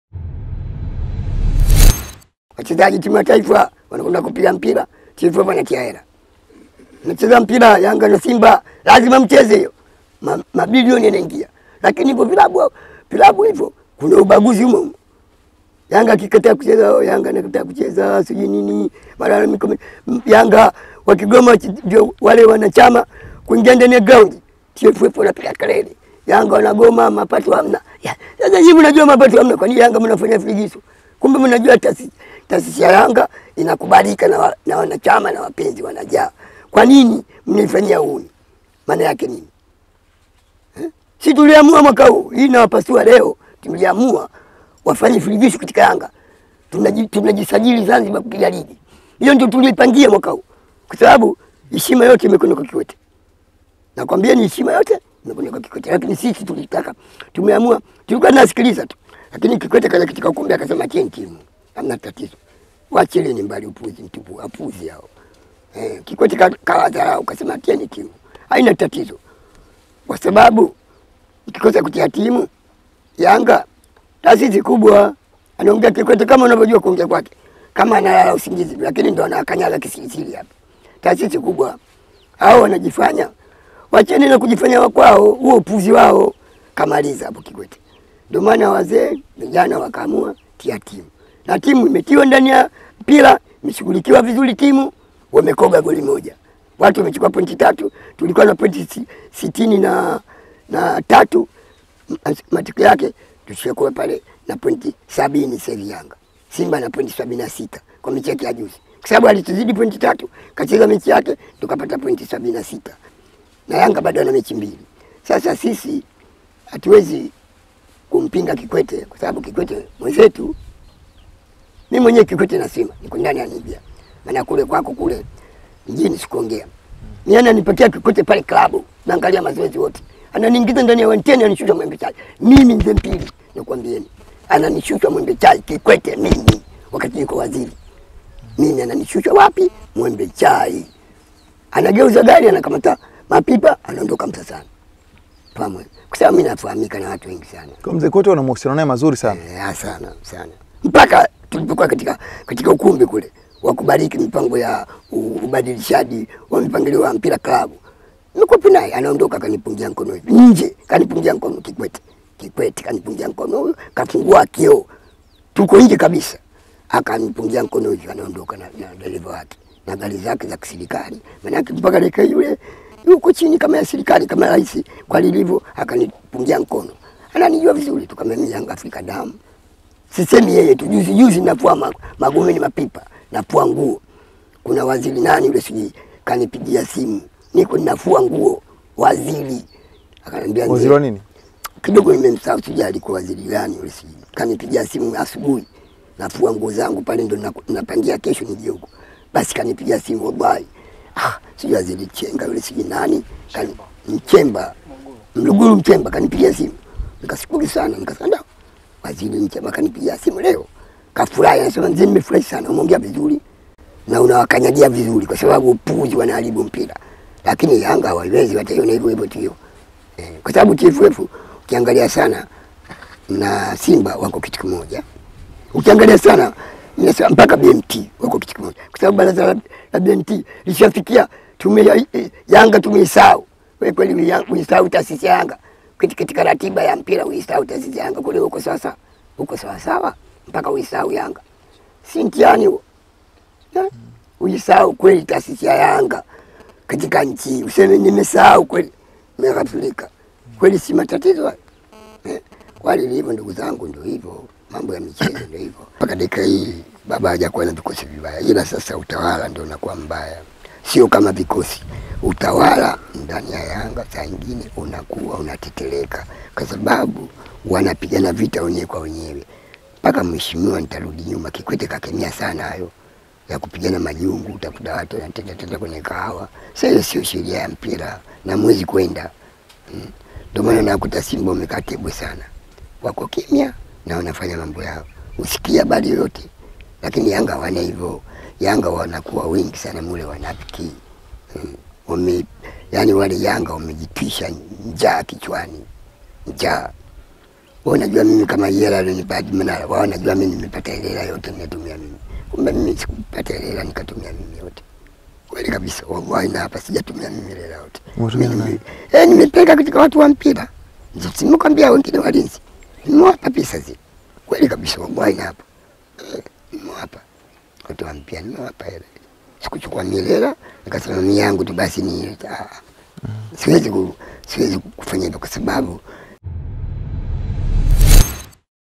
MZEE MPILI AIBUA MAPYA YA KIKWETE “HII NAWAPASUA LEO, ALINIINGIZA KWENYE GARI”
AyoTV ilifunga safari kutoka Dar es Salaam hadi Rufiji Pwani katika kijiji cha Ngomboloni ambapo ndio yalipo makazi ya shabiki na mwanachama mwandamizi wa Yanga SC Mzee Haji Omary Mpili kubwa kutaka kujua historia yake ya maisha hadi kujiunga na Yanga SC.